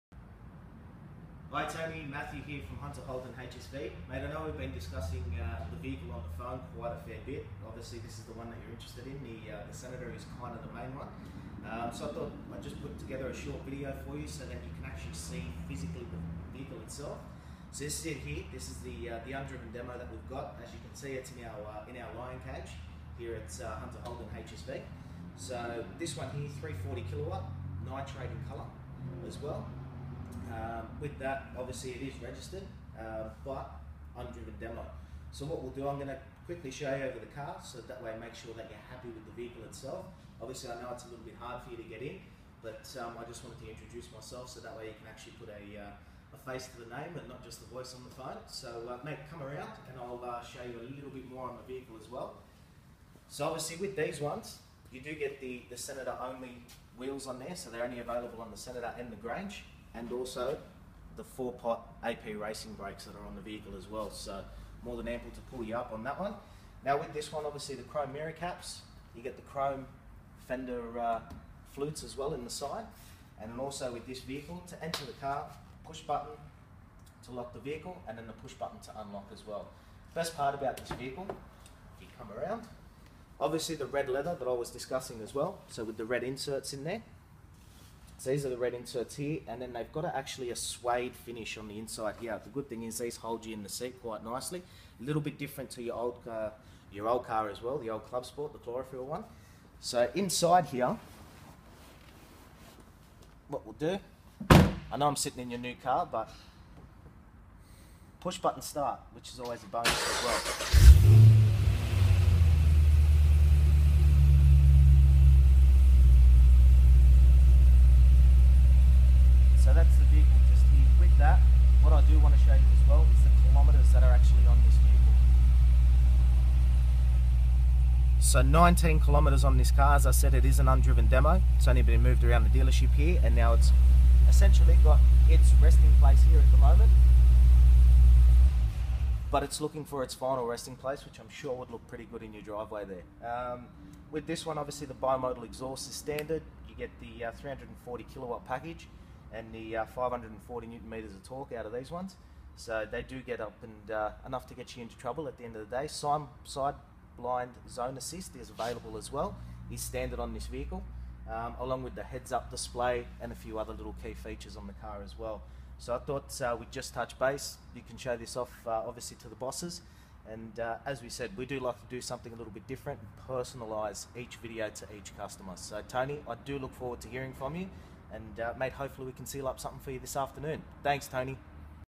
2013 HSV SENATOR SIGNATURE Vehicle Walk Around at Hunter Holden
￼2013 HSV Senator Signature ￼Matthew Porfida takes a few minutes to walk you around the 2013 HSV SENATOR SIGNATURE. Stop by our dealership for your own personal view and test drive with Matthew Porfida!